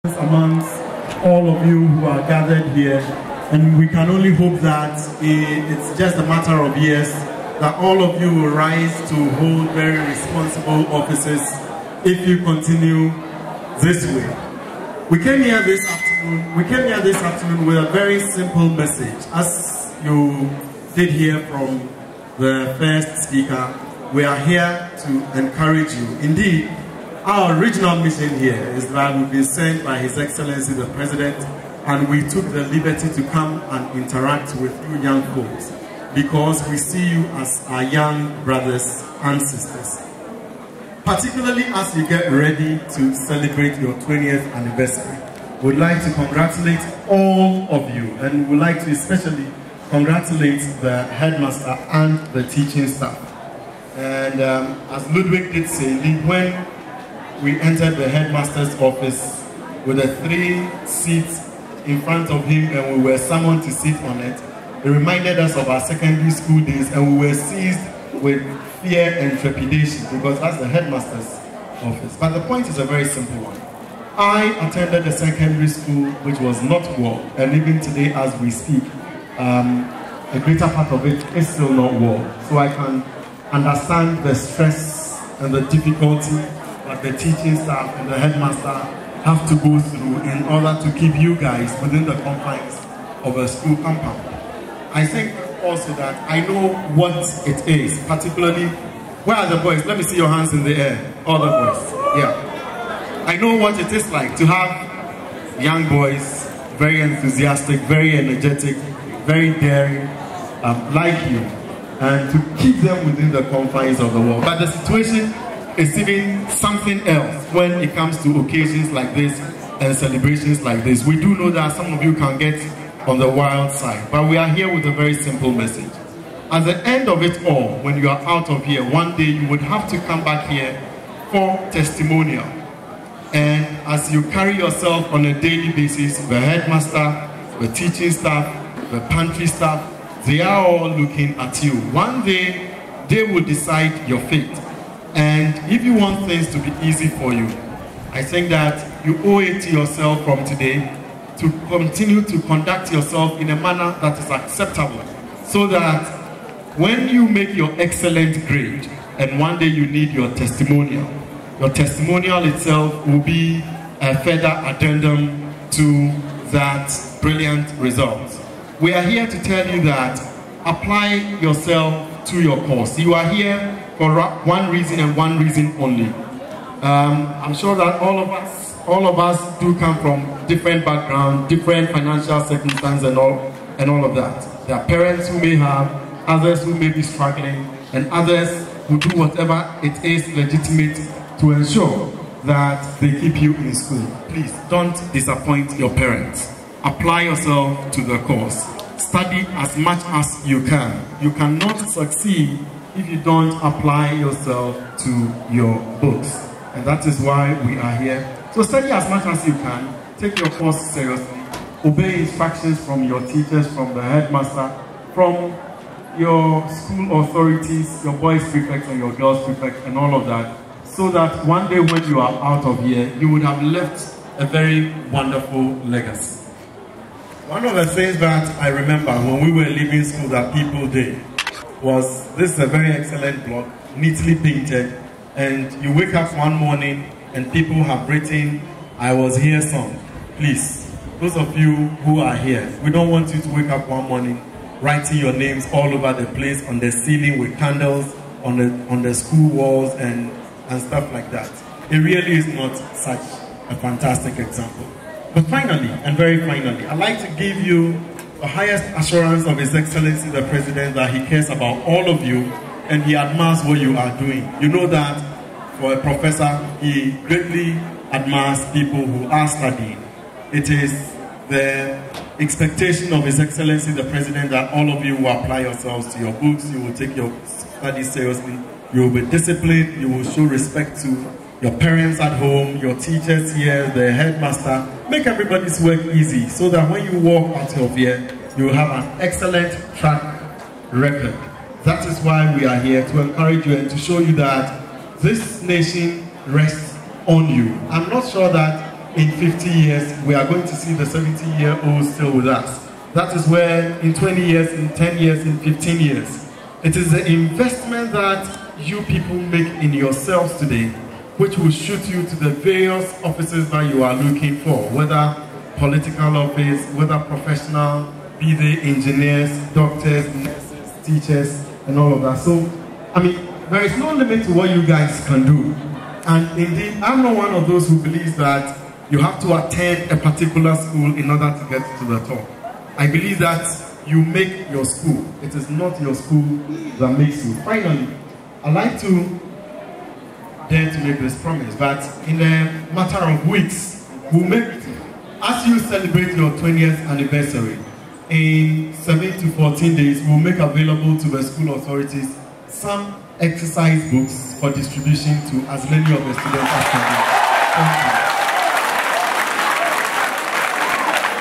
James Agyenim-Boateng, Deputy Information Minister, @ AKATSISECTEC on Volta Tour.
Deputy Minister of Information, James Agyenim-Boateng, was at AKATSI Secondary/Technical Institute to address students as part of their preparations to climax their 20 years anniversary celebrations. James Agyenim-Boateng, a lawyer by profession, is one of the young ministers appointed in the Republic of Ghana and he has served as Deputy Information and later as Deputy Tourism Minister. He is also a trained journalist and a broadcaster.
James Agyenim-Boateng was part of the team of young government appointees who accompanied Ludwig Akpene Hlodze, NDC National Youth Organiser on a 3 day working visit to the Volta region from Wednesday February 22, to Friday February 24, 2012.

Produced by Felix Mawulolo Amegashie, an NDC Youth Activist.